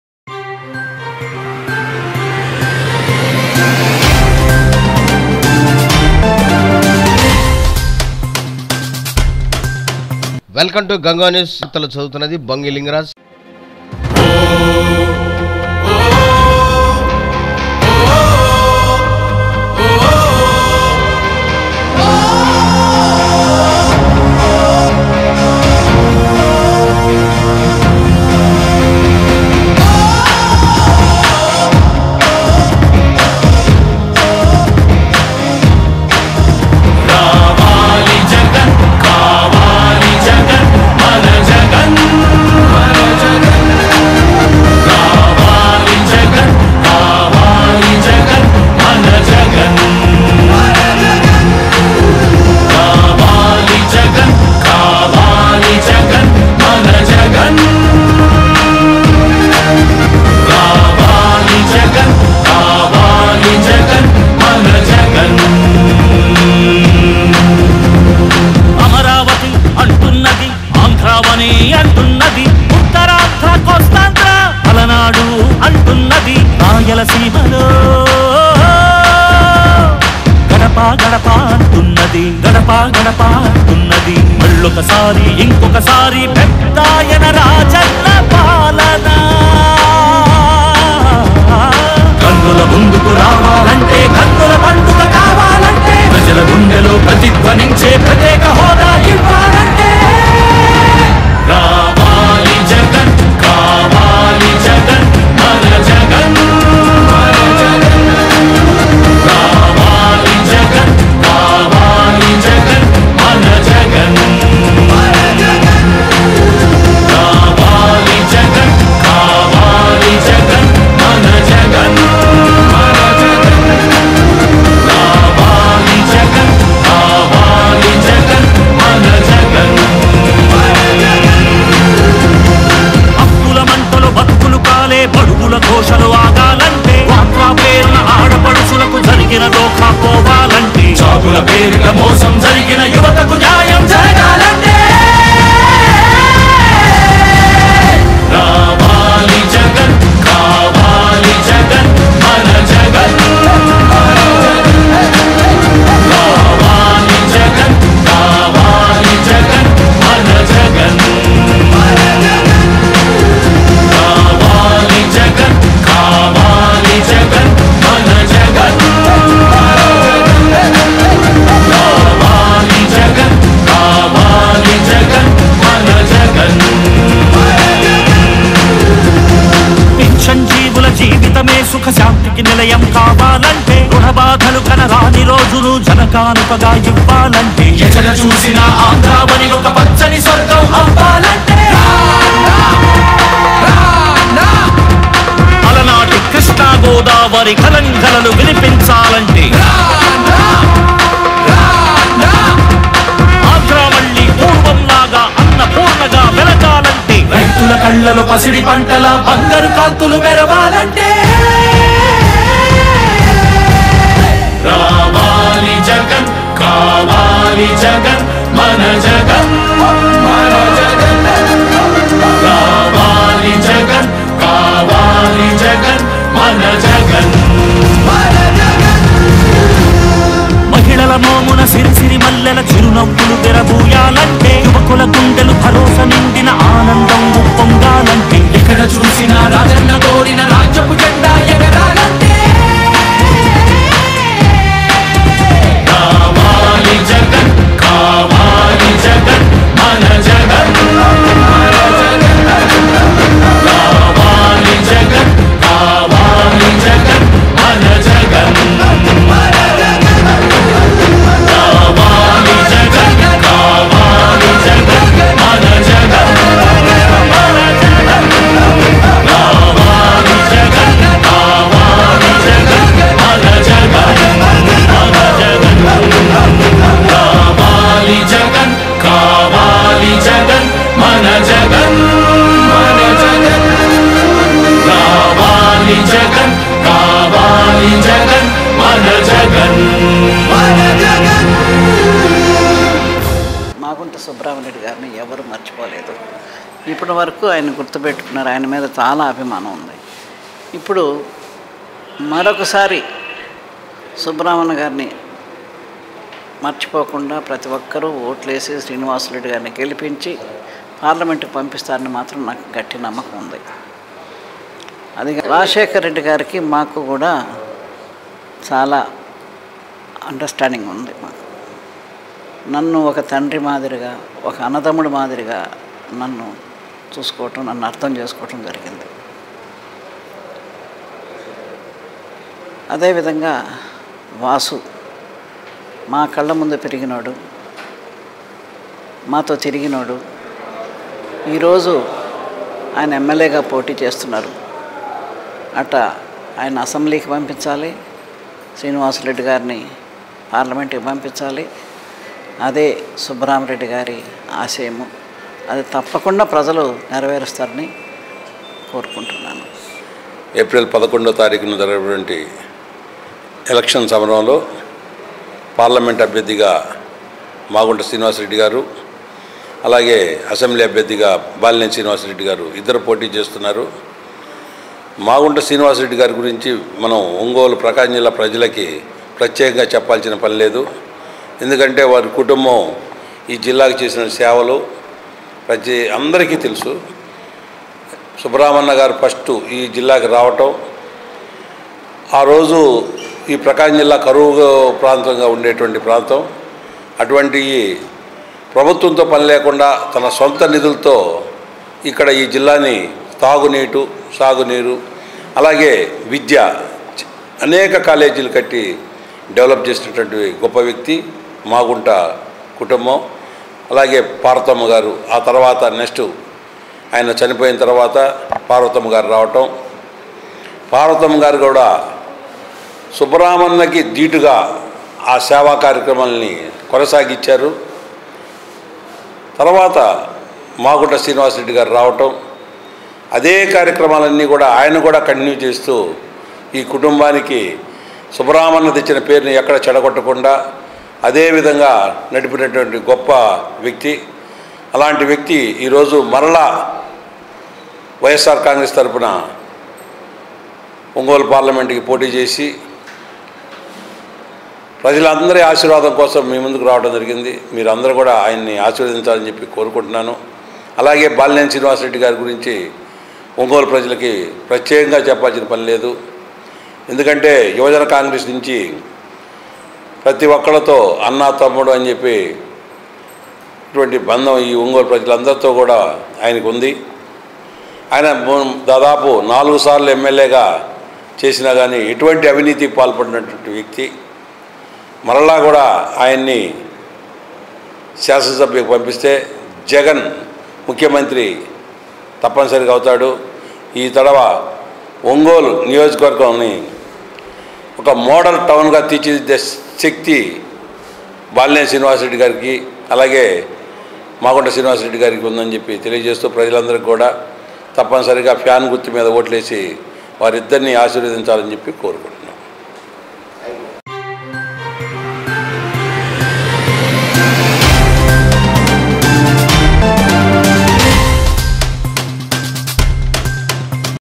ఎన్నికల ప్రచారంలో బాలినేని శ్రీనివాస రెడ్డి మాగుంట శ్రీనివాసుల రెడ్డి మాగుంట రాఘవరావు
బాలినేని ప్రణీత్ రెడ్డి మాగుంట పార్వతమ్మ తదితరులు పాల్గొన్నారు GANGA NEWS 29/3/2019